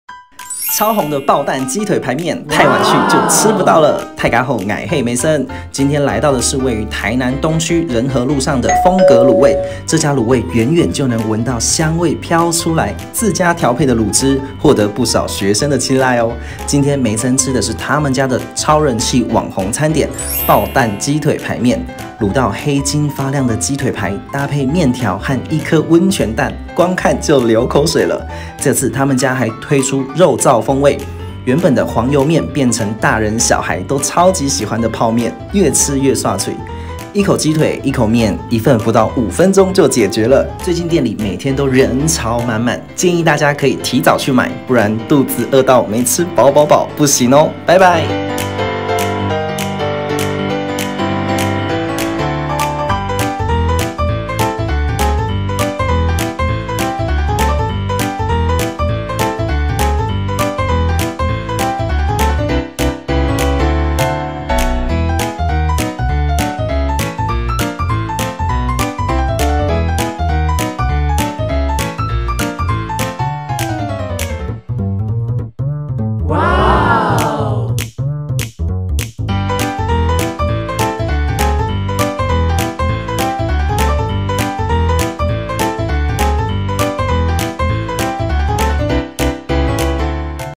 台灣台南小吃｜瘋格滷味｜🐔 台南小吃 瘋格滷味 生意怎麼這麼好？梅森Maysun
🐔 台南小吃 瘋格滷味 生意怎麼這麼好？
【 ιиfσямαтισи｜店家資訊 】
🚩 店名：瘋格滷味
📮 地址：70174台南市東區仁和路71-2號
⏰ 時間：17:15–22:00（週六、一休）
🦞 粉專： @crazystyle0925
💬 超紅的爆蛋雞腿排麵；太晚去就吃不到了
今天來到的是位於台南東區仁和路上的「瘋格滷味」這家滷味遠遠就能聞到香味飄出來。自家調配的滷汁，獲得不少學生的青睞
今天梅森吃的是他們家的超人氣網紅餐點「爆蛋雞腿排麵」
滷到黑金發亮的雞腿排，搭配麵條和一顆溫泉蛋，光看就流口水了！

這次他們家還推出肉燥風味，原本的黃油麵換成大人小孩都愛的泡麵，越吃越涮嘴
一口雞腿一口麵，一份不用5分鐘就解決了
最近每天人都好多，建議大家可以提早去買
不然肚子餓到沒吃飽飽飽不行哦！
【 мєиυ｜菜單 】
🍽 肉燥風味爆蛋雞腿排麵 ɴᴛ110
🍽 原味爆蛋雞腿排麵 ɴᴛ105
【 ¢ℓαѕѕяσσм｜梅森客棧 】
① 蕃薯 — fan+ shuˇ
② 馬鈴薯 — ma+ lin^ shuˇ
③ 傳統 — 傳統chonˇ tung^

影片創作者
🇹🇼 用脂肪和聲音影像創作的部落客家人
☀️ 心┌　　　　　　　　　　 ┐
🌤 分
⛅️ 享 肚子好餓！晚餐吃啥？
🌥 熱
☁️ 量└　　　　　　　　　　 ┘